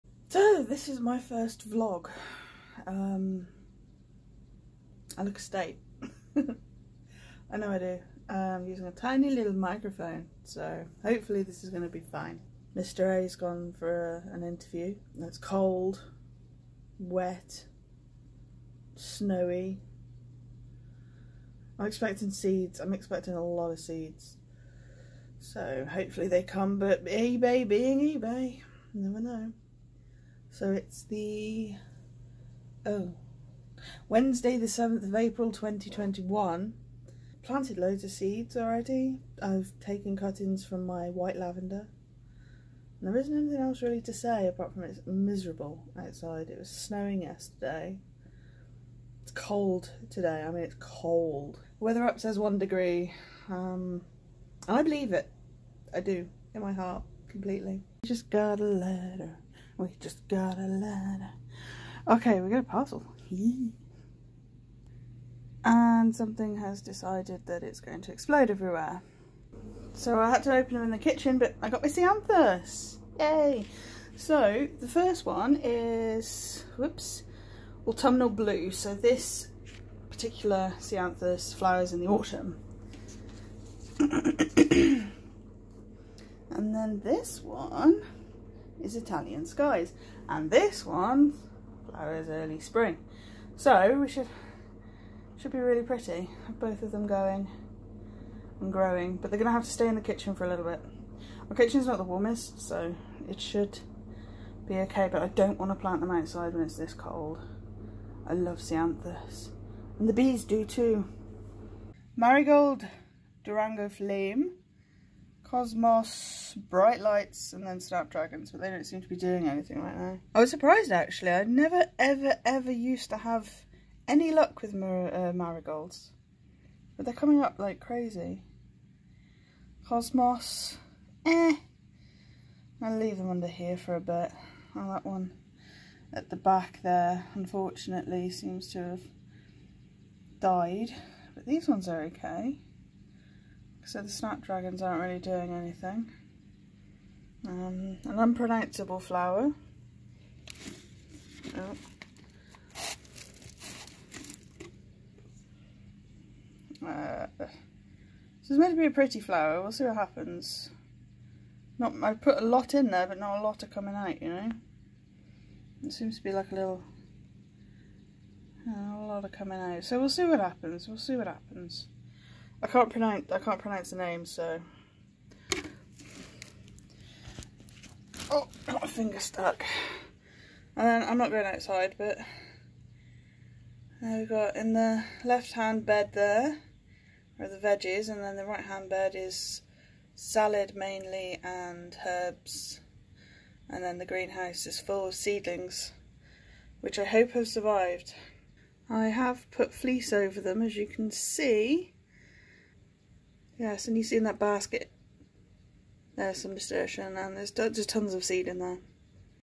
The Weather outside is FREEZING! and I hope my Seedlings survive. VLOG 1
Seeds have been planted, new bushes arrived and general thoughts on the terrible weather.

Vlog 1 of possibly many. The beginning of April 2021.

Based in Shropshire, UK